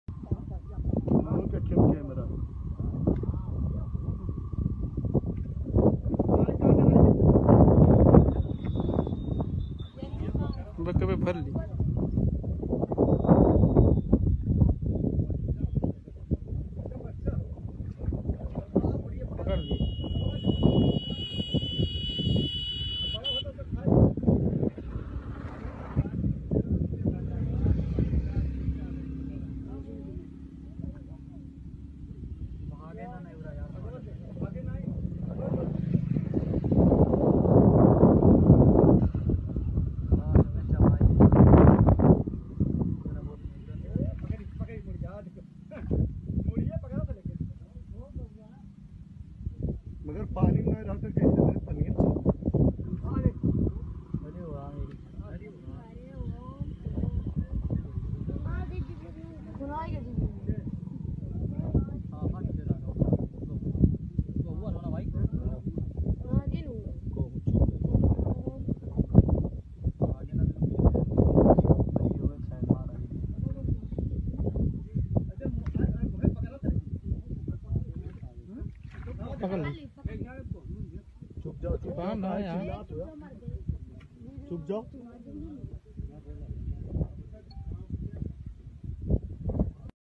Cobra and Mongoose fight
कोबरा और नेवला के बीच हुई जबरदस्त लड़ाई

Love you so much guys